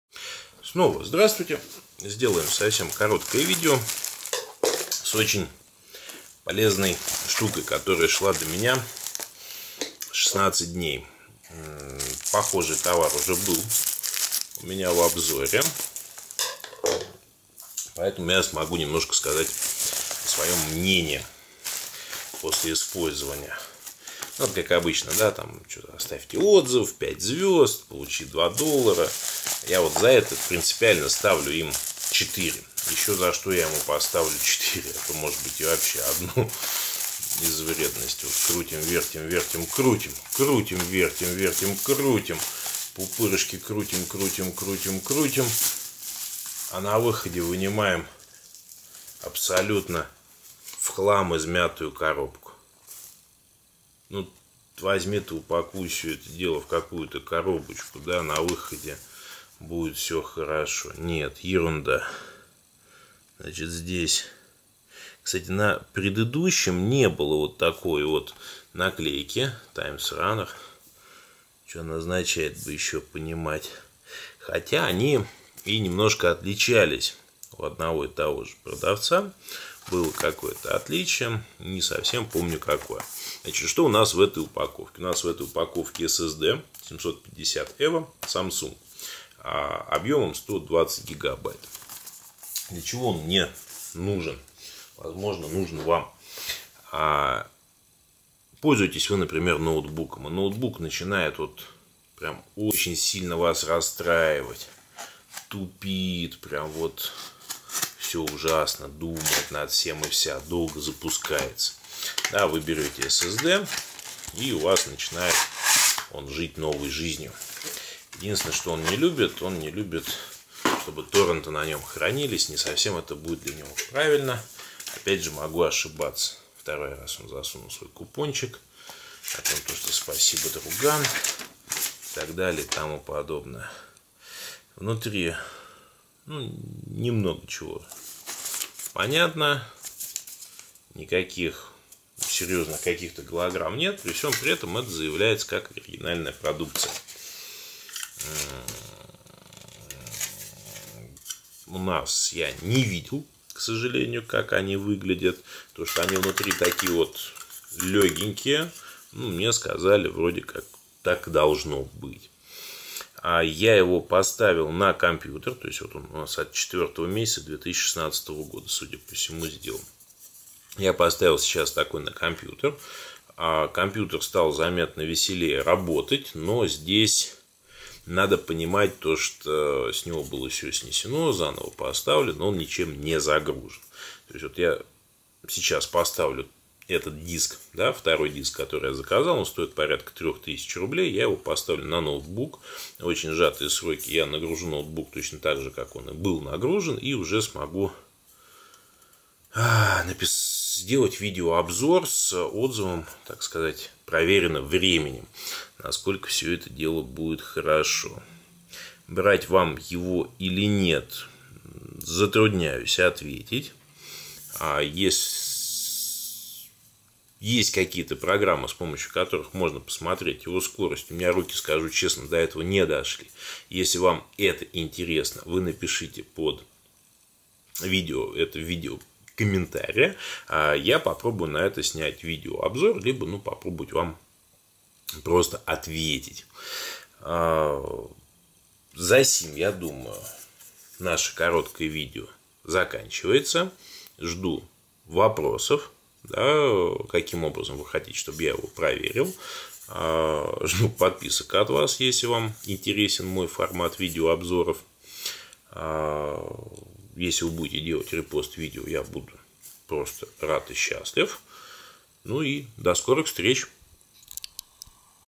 Samsung SSD 750 EVO с AliExpress Unboxing
SSD Samsung 750 EVO 120\250GB я брал 120GB. Ноутбук стал летать. Если интересуют показатели, то напишите какой программой проверить диск на характеристики.

мой кешбек с помощью которого возвращаю часть денег со всех покупок на AliExpress